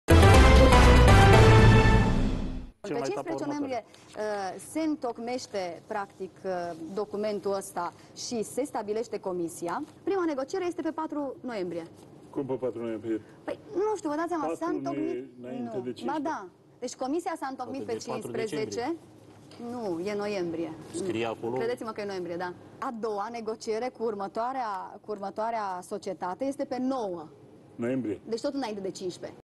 Lavinia Şandru cere demisia lui Adrian Iorgulescu
Deputatul PIN, Lavinia Şandru cere demisia ministrului Culturii Adrian Iorgulescu. Asta pentru că în toamna anului trecut, consilierul de stat Marius Marian Şolea, muzicianul Florin Chilian şi realizatorul George Mihalcea au făcut o plângere la DNA, în care reclamau ilegalităţi comise de către Ministerul Culturii şi Cultelor. Aceştia contestau modul în care zeci de miliarde de lei vechi au fost cheltuiţi pentru promovarea imaginii României în străinătate.

Conform plângerii făcută de cei trei, Adrian Iorgulescu a dat în 2005 unor apropiaţi peste 22 de miliarde de lei vechi de la bugetul de stat. Cu acesti bani trebuiau create 10.000 de pachete de promovare a României, destinate personalului diplomatic din Uniunea Europeană. Deputatul PIN susţine că firmele care ar fi trebuit să se ocupe de editarea acestor pachete au fost selectate înainte de eliberarea documentaţiei oficiale a Guvernului.